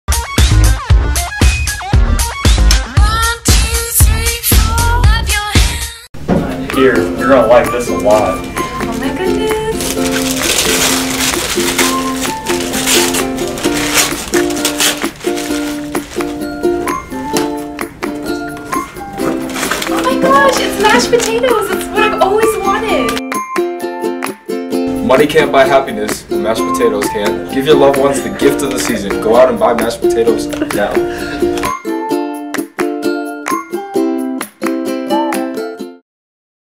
Mashed Potato Commercial
For my Digital Video and Audio class we had to create an edible commercial. I hope you guys enjoy this mashed potatoes commercial!